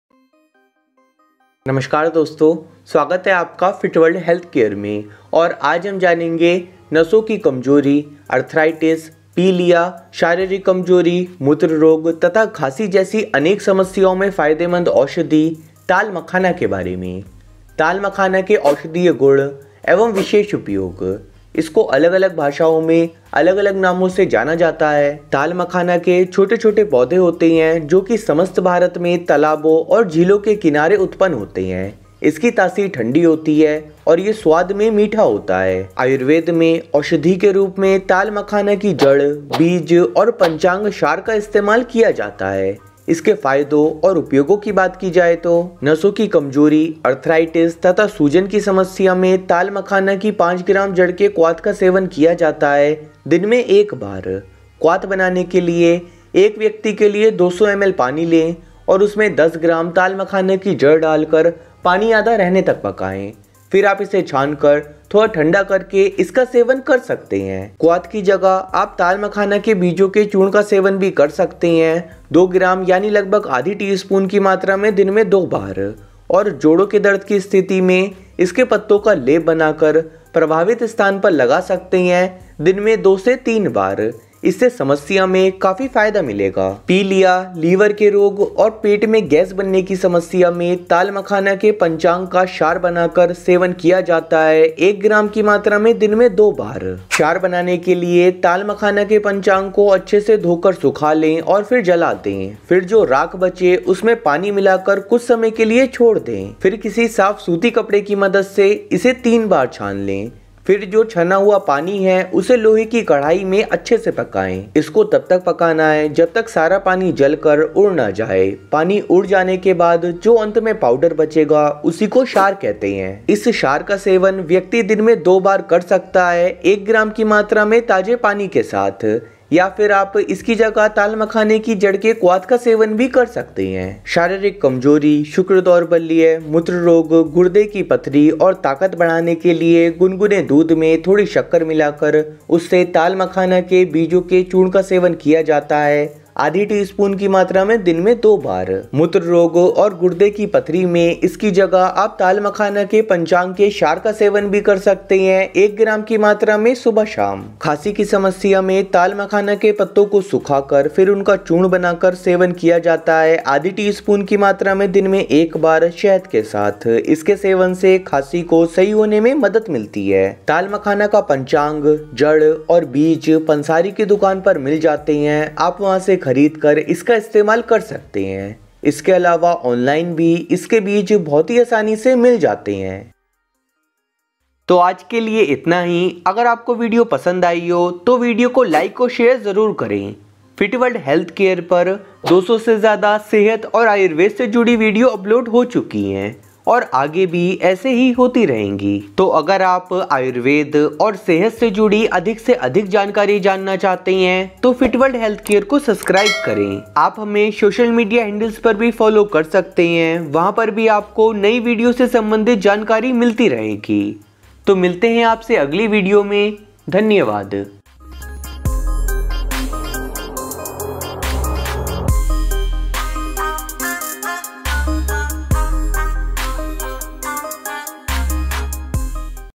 Talmakhana ke fayde | तालमखाना खाने के फायदे | Hygrophila spinosa | asteracantha longifolia
✨️अगर आपको इस वीडियो से संबंधित कोई भी सवाल है तो आप कमेंट सेक्शन में पूछ सकते हैं

✨️If you have any question related to this video then you can ask in comment section

🌟 तालमखाना / Hygrophila spinosa / Asteracantha longifolia

इस वीडियो में आपको जानने को मिलेगा तालमखाना के औषधीय गुण एवं विशेष उपयोग

In this video you will get to know the medicinal properties and special uses of Asteracantha longifolia

👉 Image credit

👉तालमखाना के अन्य नाम

कोकिलाक्ष, काकेक्षु, इक्षुर, क्षुरक, भिक्षु, काण्डेक्षु, इक्षुगन्धा, इक्षुबालिका, कुलियाखारा, कुलेखाड़ा, एखरो, तालीखाना, कोल सुन्दा, गोलिमिडि केट्टु, निर्गुविवेरु, कुलुगोलिके, बलिकेल, निरमुल्ली आदि

@Fit world Health care